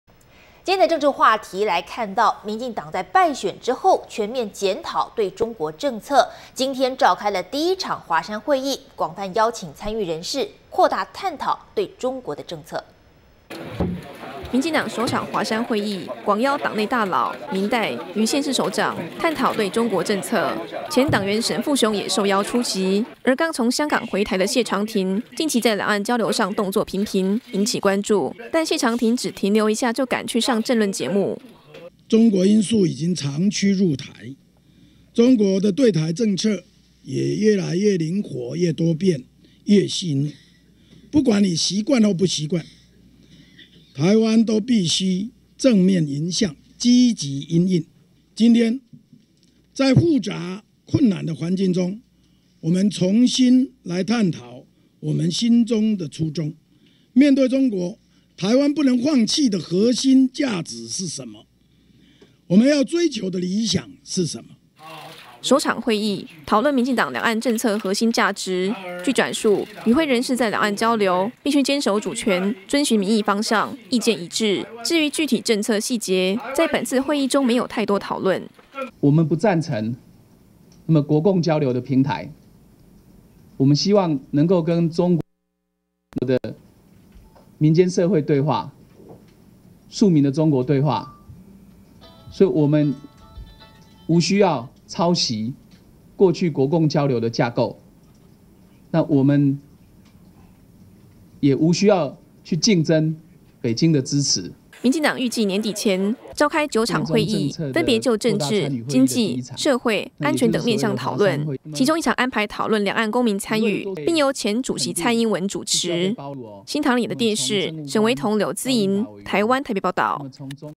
【台灣新聞】民進黨華山會議 討論對中政策
【台灣新聞】民進黨華山會議 討論對中政策(NTDAPTV) 今天的政治話題，民進黨敗選後，全面檢討對中政策。今天(4日)召開首場華山會議，廣泛邀請參與人士，擴大探討對中國的政策。【新唐人亞太台2013年7月4日訊】

民進黨首場華山會議，廣邀黨內大老，民代，與縣市首長，探討對中政策。前黨員沈富雄也受邀出席。而剛從香港回台的謝長廷，近期在兩岸交流上動作頻頻，引起關注，但謝長廷只停留一下，就趕去上政論節目。

民進黨主席 蘇貞昌：「中國因素已經長驅入台，中國的對台政策也越來越靈活越多變越細膩，不管你習慣不習慣，台灣都必須正面迎向，積極因應，今天在複雜困難的環境中，我們重新來探討我們心中的初衷，面對中國，台灣不能放棄的核心價值是甚麼，我們要追求的理想是甚麼？」

首場會議，討論民進黨兩岸政策核心價值，據轉述，與會人士在兩岸交流，必須堅守主權，遵循民意方向，意見一致。至於具體政策細節，在本次會議中沒有太多討論。

民進黨中國事務委員會發言人 鄭文燦：「我們不贊成國共交流的平台，我們希望能夠跟中國的民間社會對話，庶民的中國對話，所以我們無需要抄襲，過去國共交流的架構，那我們也無需要去競爭北京的支持。」

民進黨預計年底前，召開九場會議，分別就政治、經濟、社會、安全等面向討論。其中一場安排討論兩岸公民參與，並由前主席蔡英文主持。